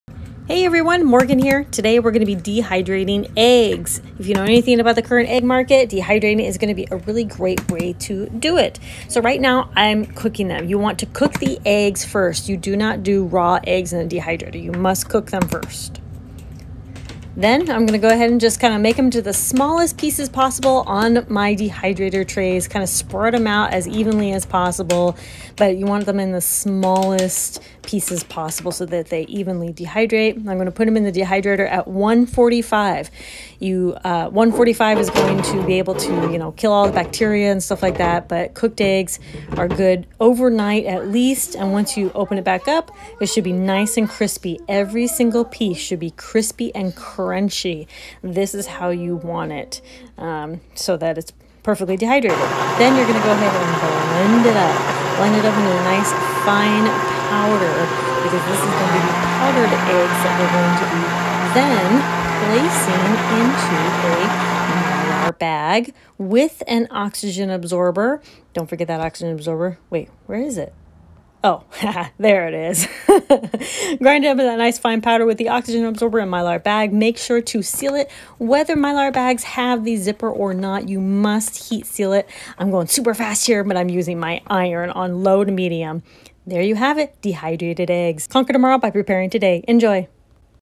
How to Dehydrate Eggs & How to Store Them
Here's a quick guide on how to dehydrate eggs at home.

Mail:
Morgan Rogue
Trapper Creek, AK 99683